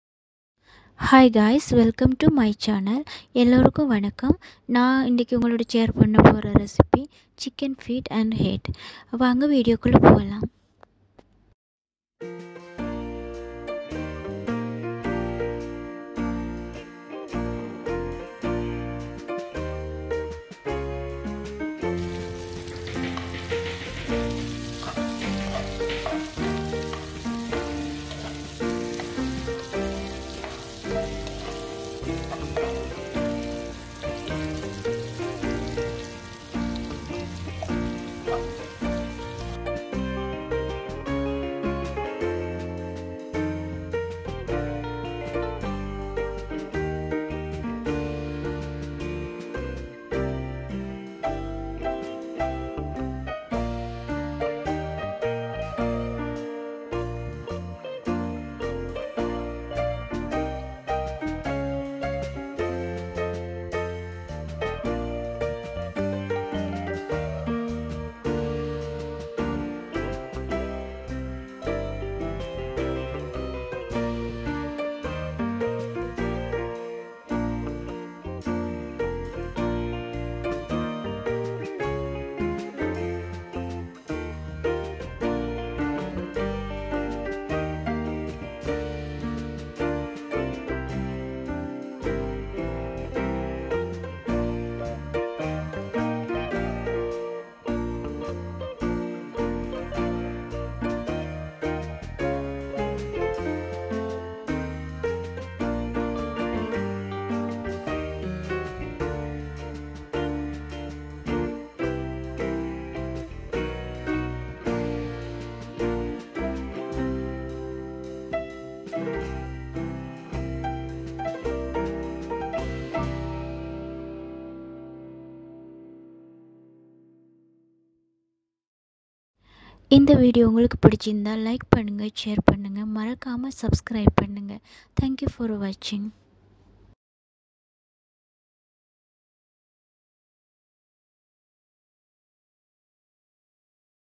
கோழி கால் மற்றும் தலை curry#Chicken feet and head#
Ingredients
Chicken feet and head 1/2 kg
Oil
Garlic paste 1teaspoon
Ginger paste 1teaspoon
Chicken masala 1 table spoon
Turmeric power 1/2 tea spoon
Cumin seed  1 tea spoons
Black paper seed Little
Bay leaves 2 pieces
Salt
Water